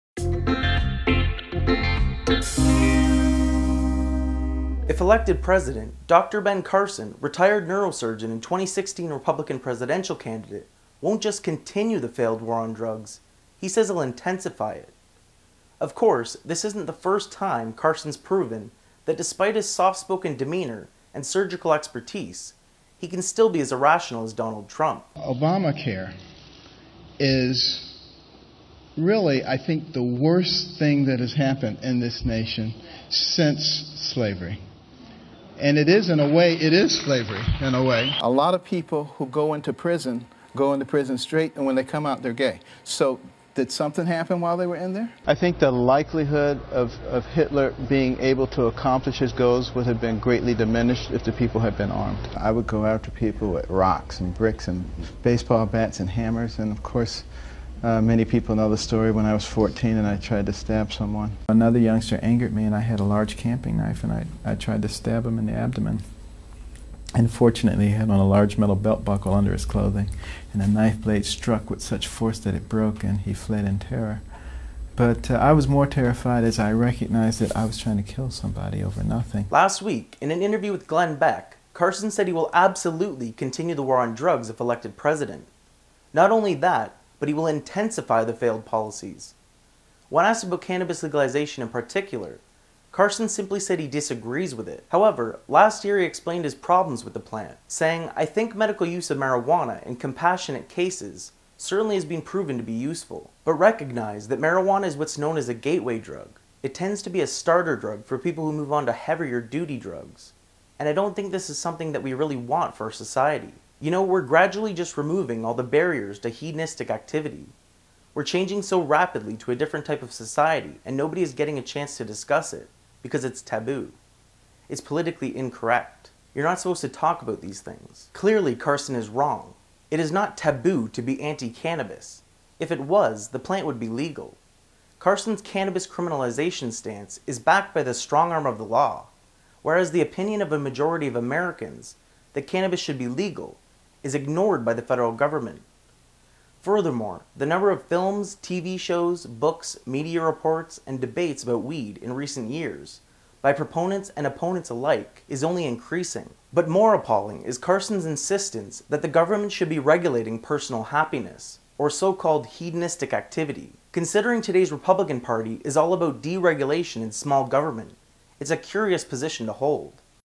Ben Carson Will 'Intensify' the War on Drugs
Dr. Ben Carson, retired neurosurgeon and 2016 Republican presidential candidate, says he would "intensify" the war on drugs.